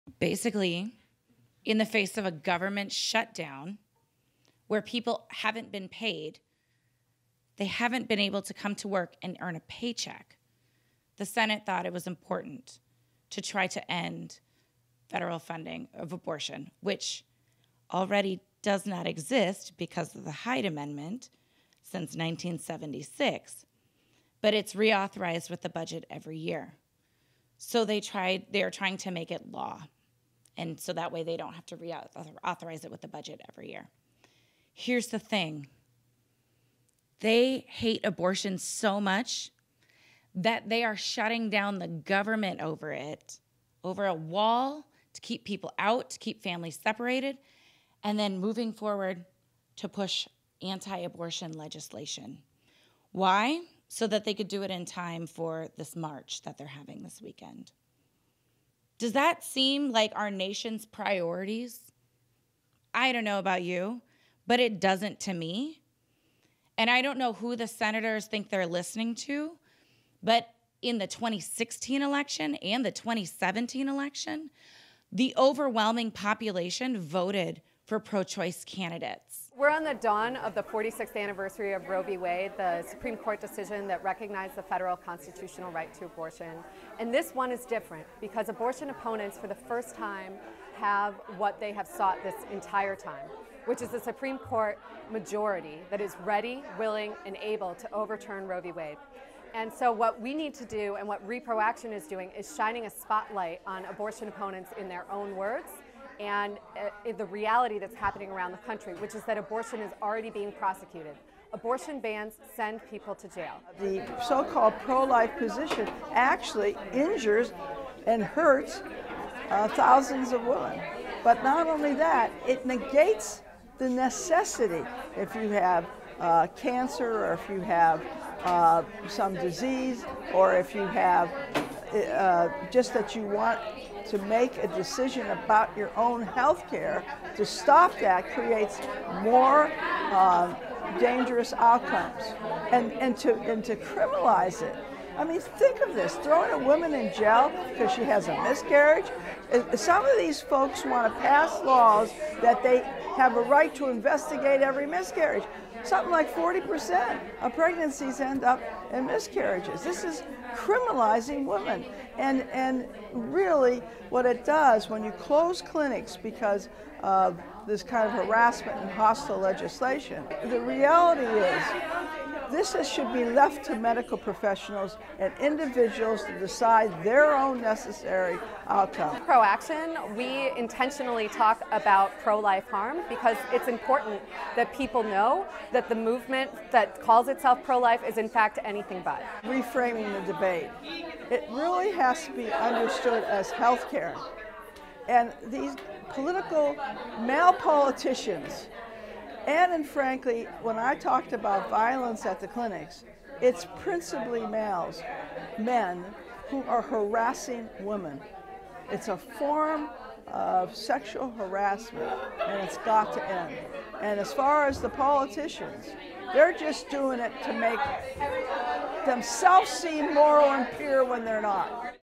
Reproaction: Stop Prosecuting Abortion
To The Contrary panelist and Co-Founder of Reproaction, Erin Matson, announced an initiative pushing to stop prosecuting abortion. Feminist Majority's Eleanor Smeal and the National Network of Abortion Funds' Renee Sherman spoke on the importance of this issue especially during the government shutdown.